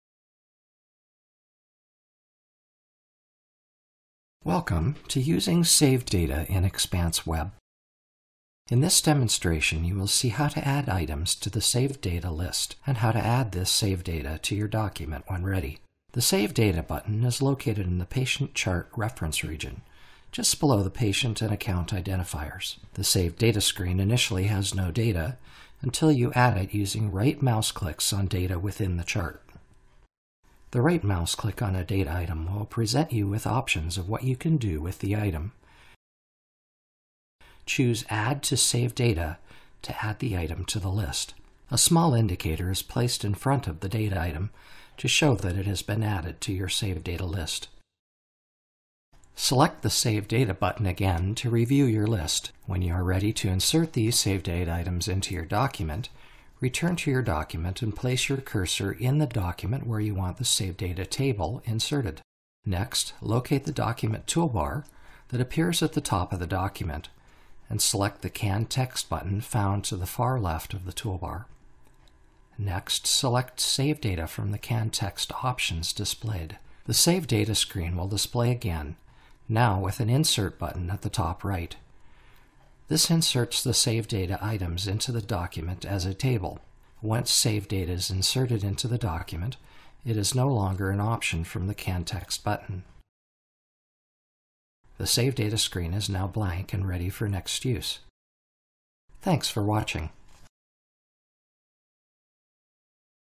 Using Saved Data with Expanse Web
Demonstration of using the Saved Data Feature in Meditech Expanse Web to collect discrete data from the patient chart and add it to your document when ready.